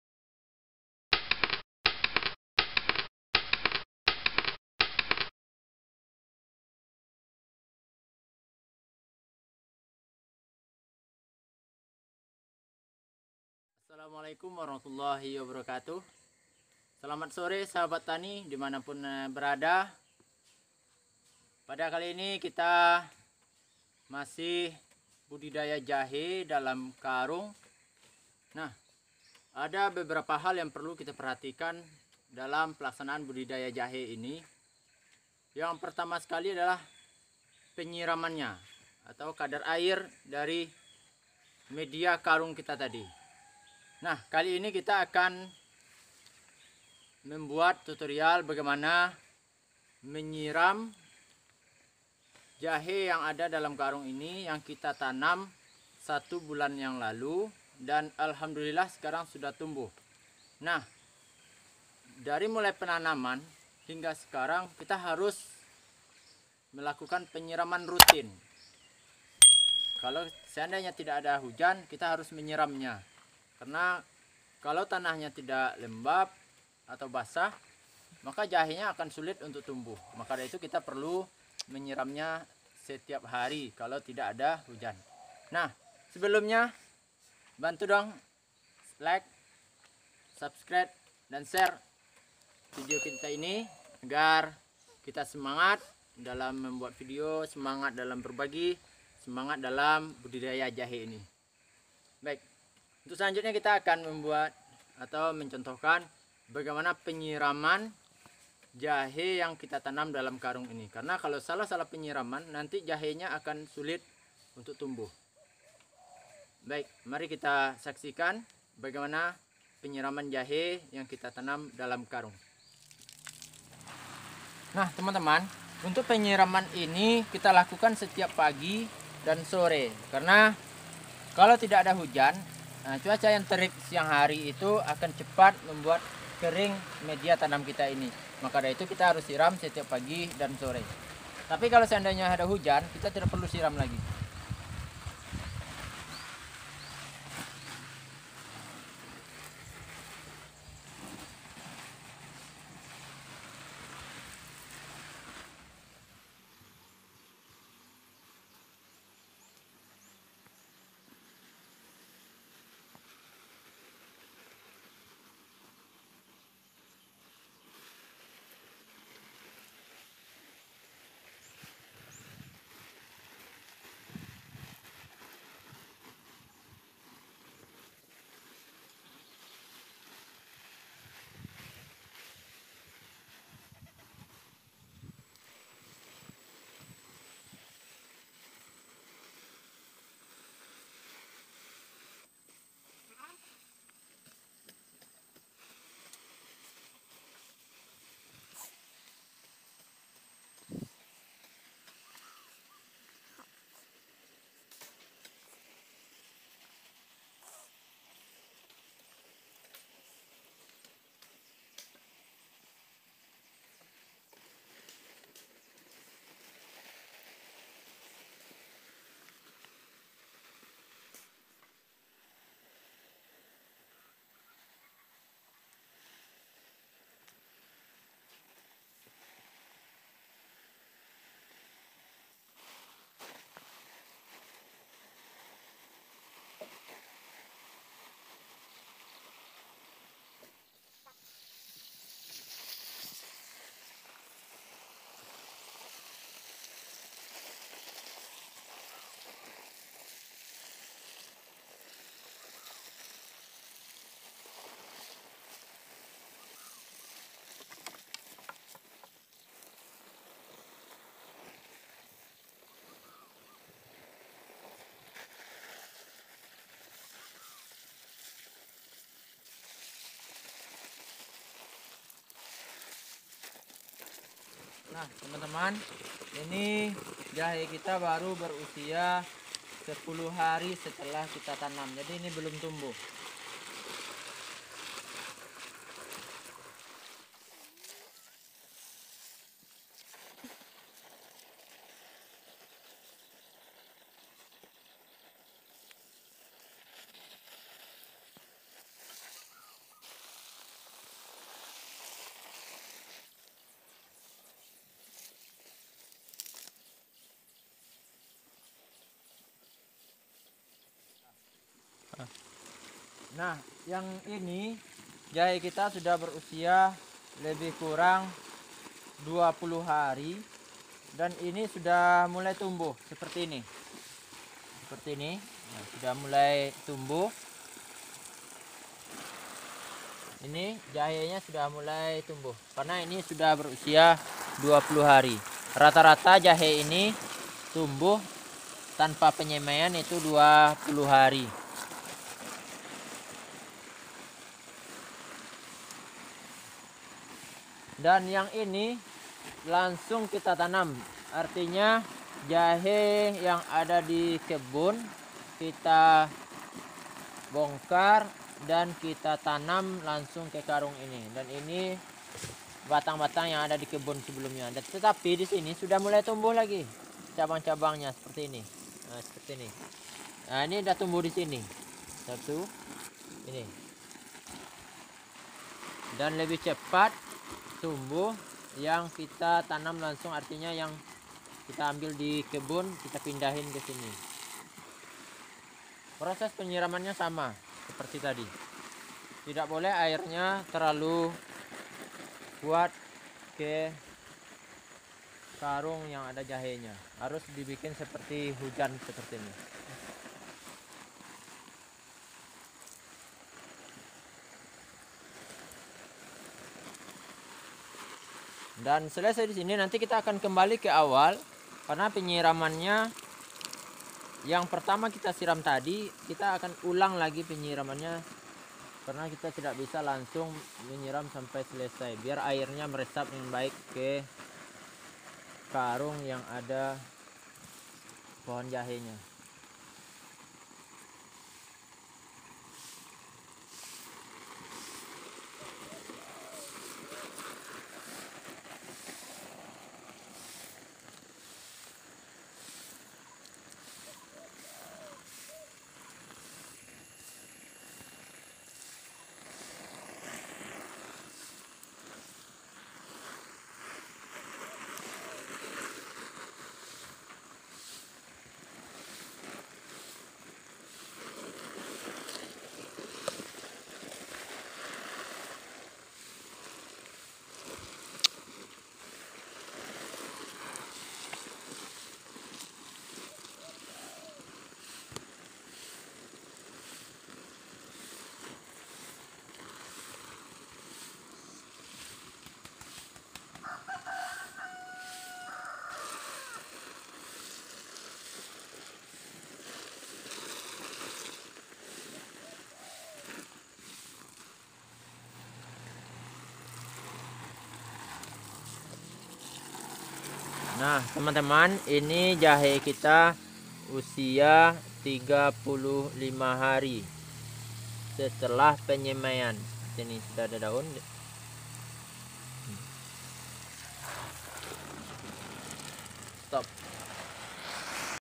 Cara mudah Perawatan jahe dalam karung agar tumbuh subur
Hal yang perlu diperhatikan saat budidaya jahe ialah perawatan. Perawatan perlu dilakukan mulia dari persiapan media tanam hingga panen. Salah satu perawatan yang perlu rutin dilakukan ialah penyiraman karena tanaman jahe tidak bisa tumbuh dengan baik jika media tanamnya kering. Berikut saksikan video penyiraman jahe yang benar